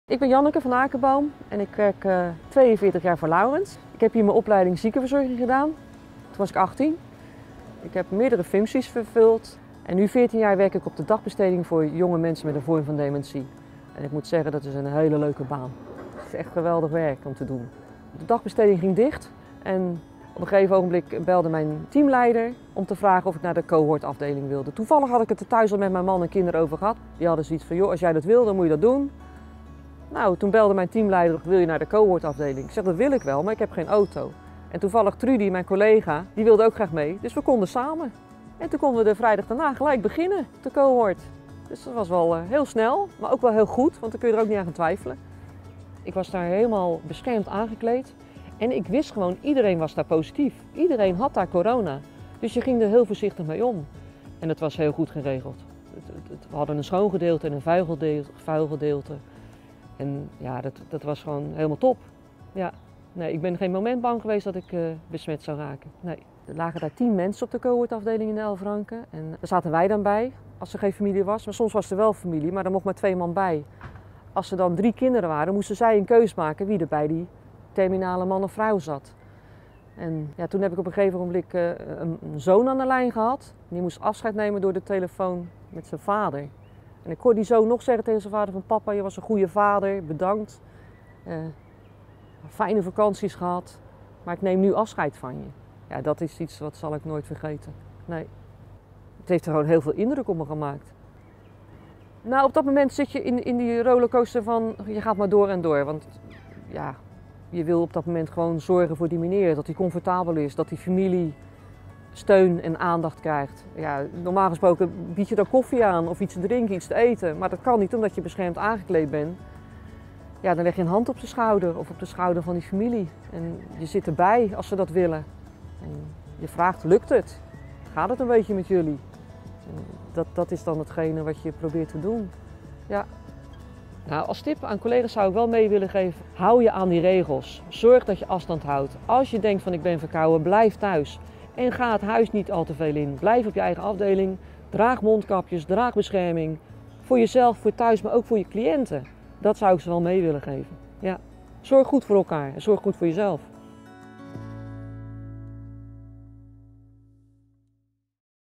Laurens is JANNEKE
Collega Janneke sprong bij op de cohort-afdeling. Het maakte heel veel indruk op haar. Zoals de zoon die afscheid nam van zijn vader door de telefoon. 'Op zo'n moment ga je zelf maar door en door, en ben je met name bezig met de zorg voor die meneer en zijn familie.'